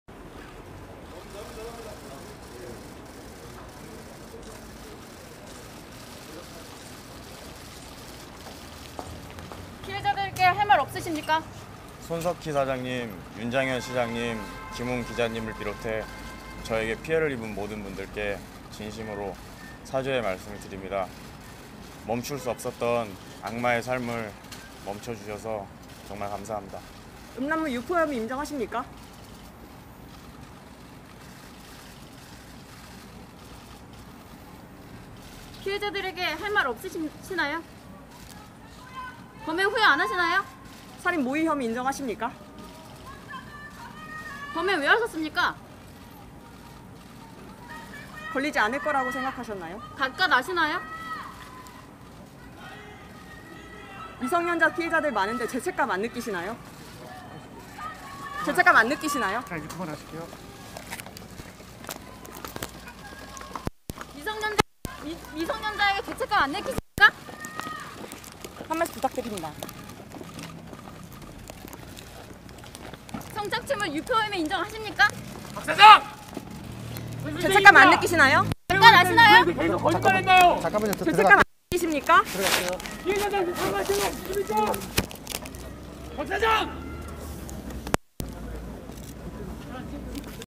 "악마의 삶 멈춰줘서 감사하다"···조주빈이 얼굴 드러내고 한 말
25일 오전 서울지방경찰청은 미성년자를 포함한 여성들의 성 착취물을 제작·유포한 혐의로 구속된 혐의로 조주빈을 기소 의견으로 검찰에 송치했다.